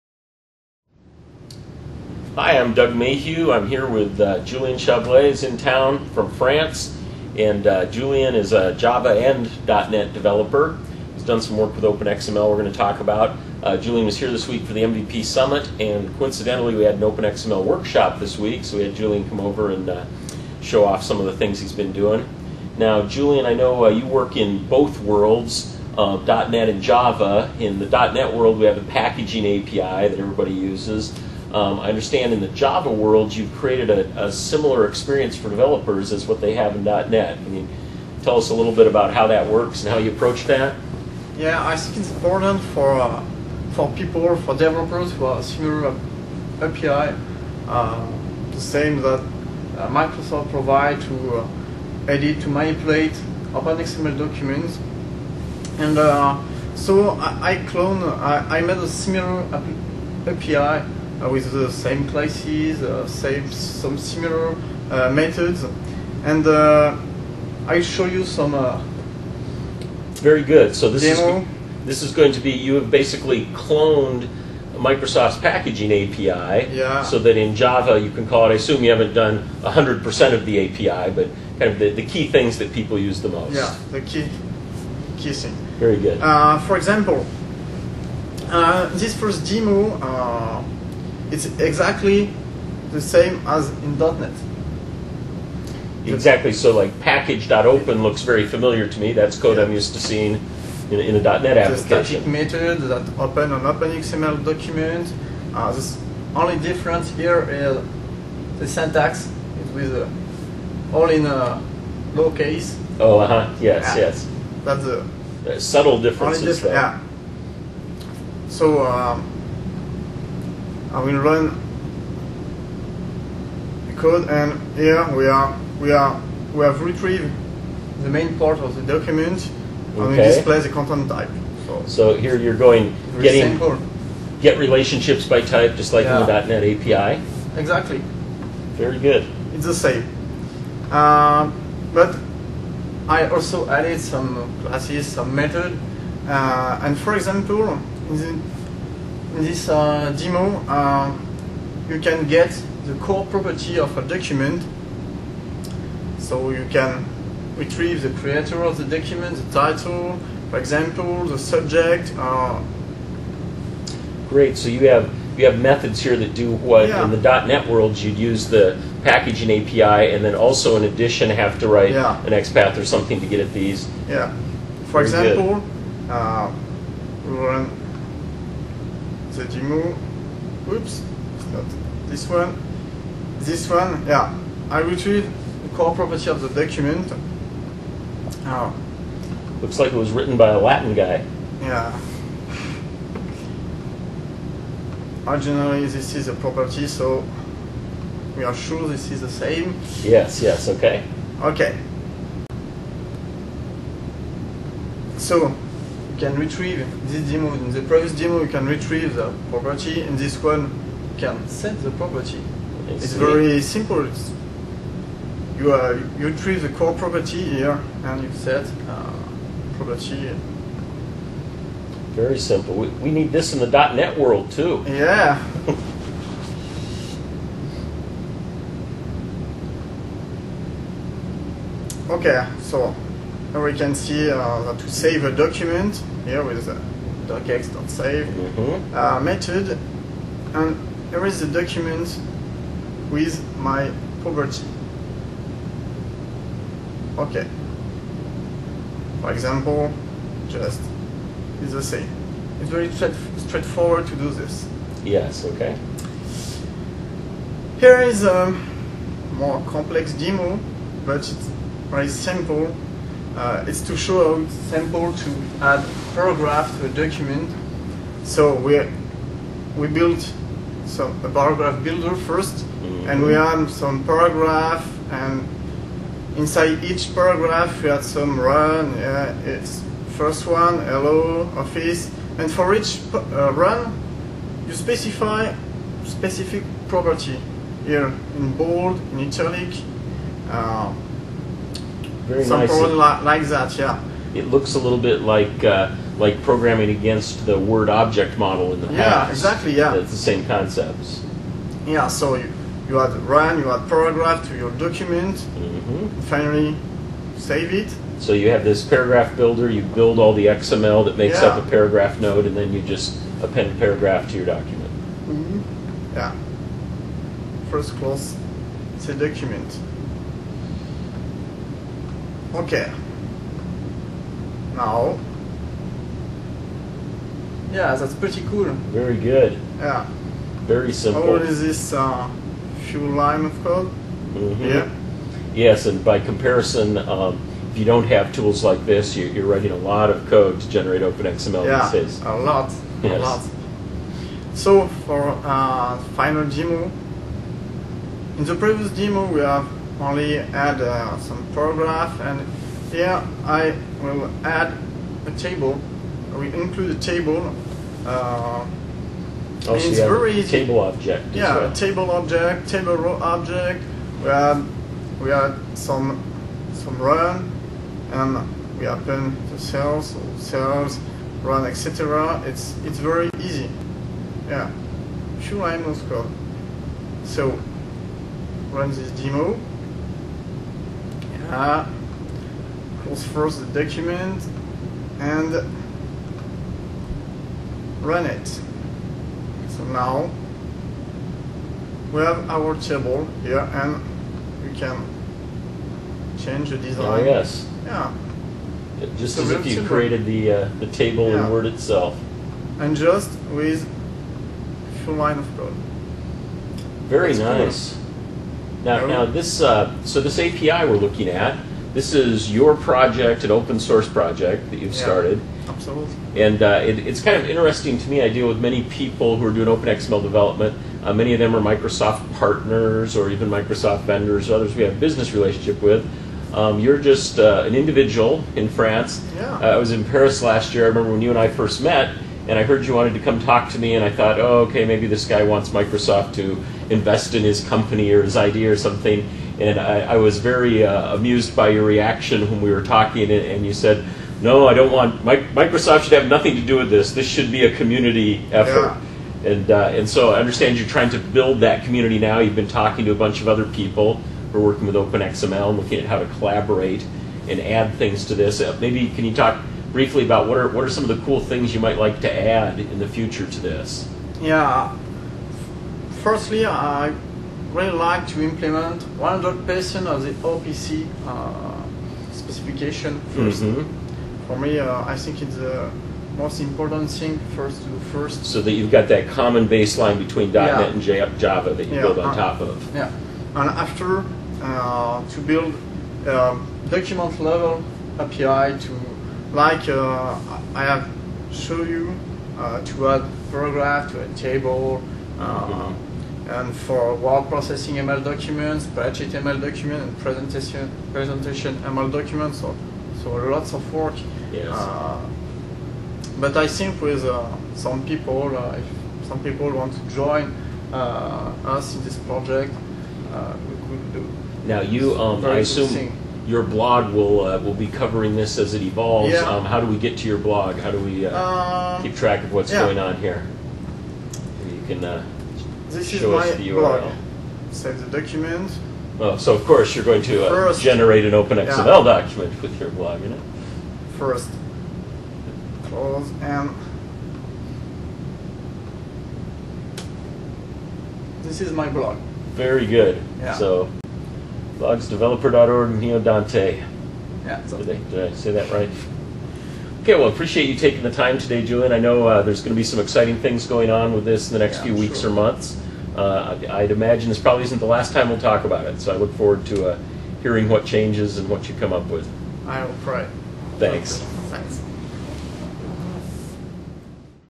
Open-source Open XML API for Java
Julien Chable has been working on an open-source API project that would give Java developers a tool for rapid development of Open XML solutions. Since the Java environment doesn't have the .NET Framework 3.0 packaging API (System.IO.Packaging) to support the low-level details, the first phase of this project is to clone the packaging API in Java, which is an ambitious task. Nonetheless, Julien has already delivered the basics of iterating through package parts and writing documents, and he is now looking for contributors to expand the project significantly in the months ahead.

Julien was in Redmond for the MVP Summit, and Doug Mahugh caught up with him in Building 20 to discuss the status of his API project and look at some hands-on example of how it works. Julien demonstrates the Java packaging API, as well as some higher-level objects he has built on top of that layer, and describes his goals for the project. If you're a Java developer with an interest in Open XML, see the SourceForge link below for information on how to participate in this project.

Mon, Mar 19, 2007